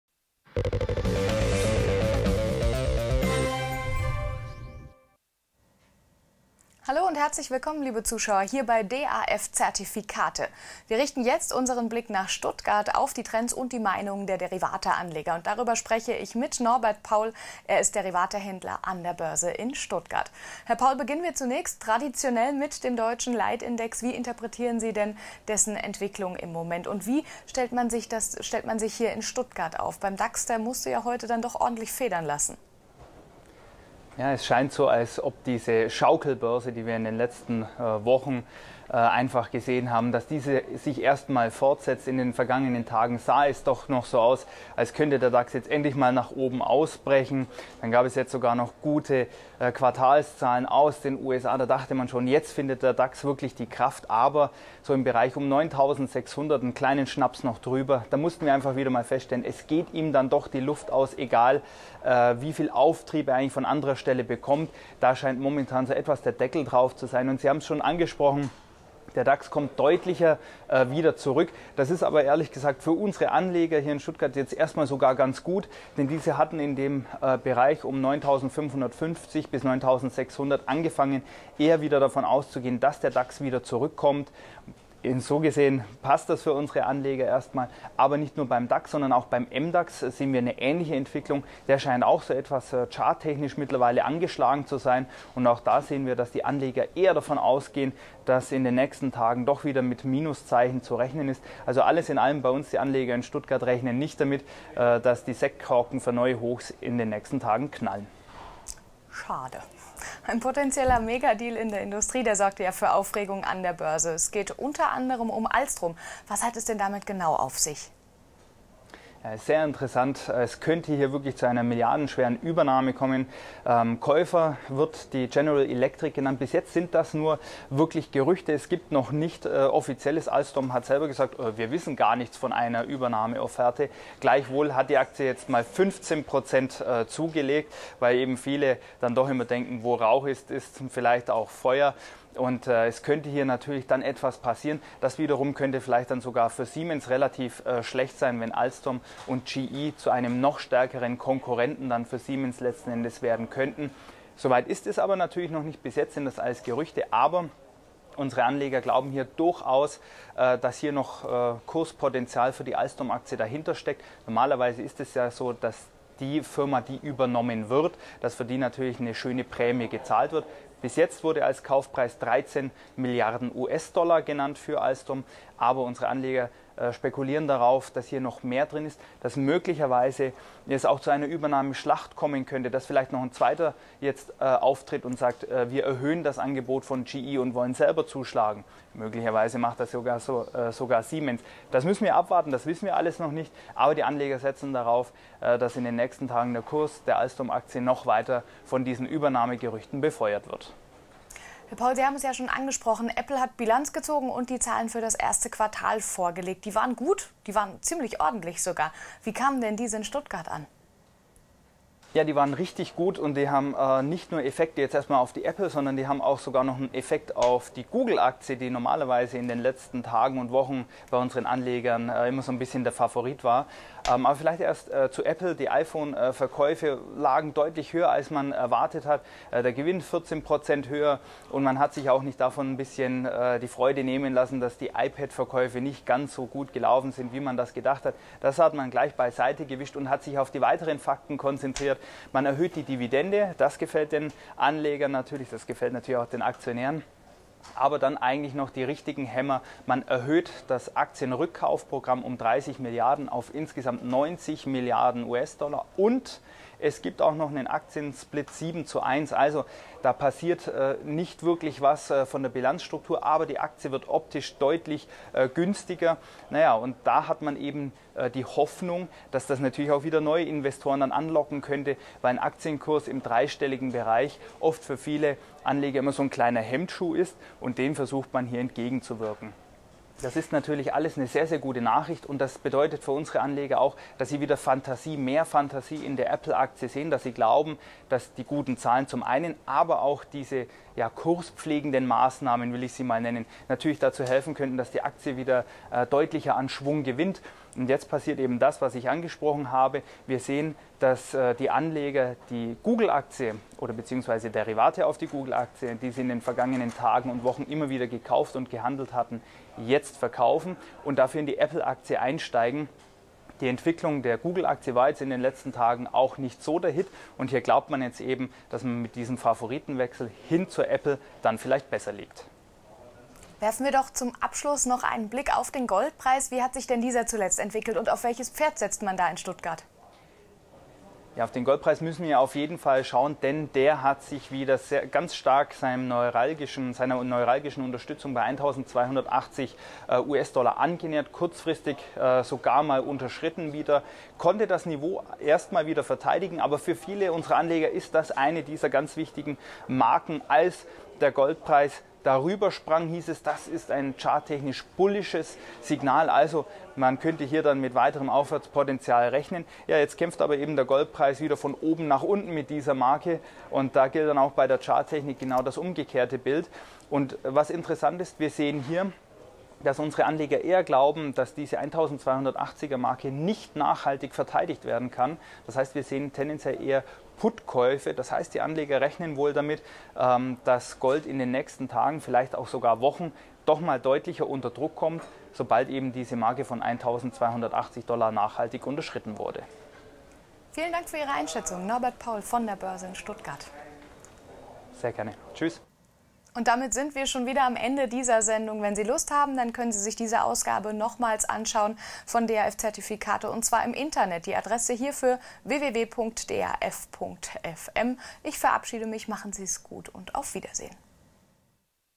DAF Zertifikate: Apple-Aktie versprüht wieder Charme - Wichtige Marke bei Gold
In DAF Zertifikate beschäftigt sich Norbert Paul, Derivatehändler an der Börse Stuttgart, mit der volatilen Verfassung, die der DAX immer wieder an den Tag legt. Aus seiner Sicht werden die Sektkorken erst einmal nicht knallen für den deutschen Leitindex. Zu den weiteren Themen gehören Alstom, Apple und der Goldpreis.